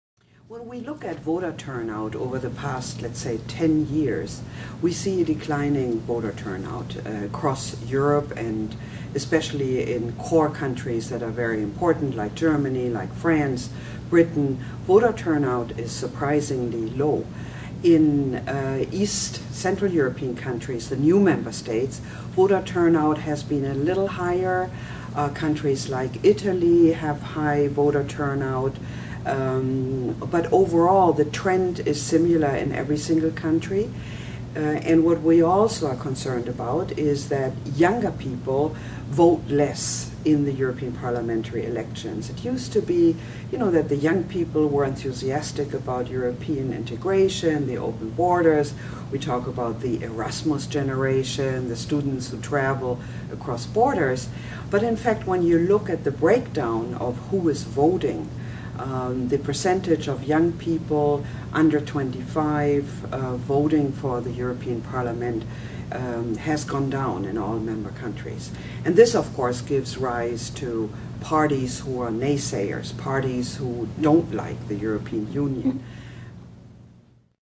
Interview: Christiane Lemke, France & the European Elections 2
On May 16, the French-American Foundation hosted a panel discussion, France and the European Elections,to discuss the upcoming European parliamentary elections, to be held  on May 25, 2014. The discussion included panelists Christiane Lemke, Max Weber Chair in German and European Studies, New York University, Martin Schain, Professor of Politics at New York University, and Emmanuel Saint-Martin, Correspondent at France 24 and Founder and CEO of French Morning.

Prior to the panel, Christiane Lemke joined the Foundation to answer a few questions about the upcoming elections.

Question 2: This seems very similar to the situation in France, where there is minimal interest and very low voter turnout. As the two largest economies in Europe, do France and Germany see these as less important because of their prominence within the European Union? Do the European elections incite greater interest in smaller EU nations?